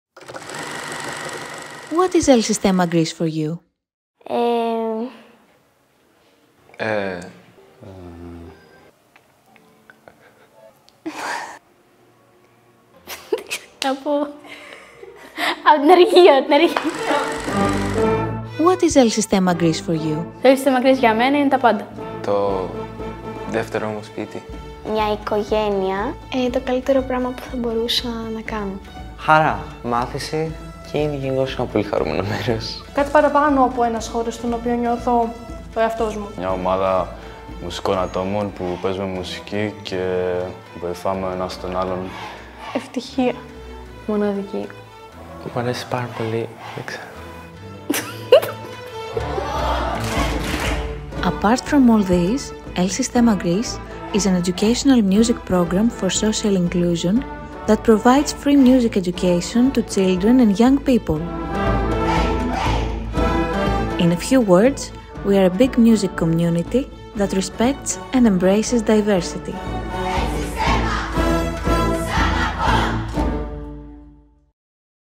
El Sistema Greece | Educational Music Program for Social Inclusion
What is El Sistema Greece for you?

“My second home”
“A family”
“More than a place that I feel myself”

We asked our musicians what El Sistema Greece means to them, and they captured the identity of our big and vibrant music community. Through their words they describe everything we do and everything we never imagined we could do through music.
El Sistema Greece is all these faces and many more not shown in this video, who always find the best way to talk about who we are.
El Sistema Greece is an educational music program for social inclusion that offers free music education to children and young people in order to create a music community, without restrictions and discrimination. El Sistema Greece's music ensembles offer a unique group music learning process, based on the principles and innovative teaching practices of El Sistema Venezuela.

Video: Ioanna Spanou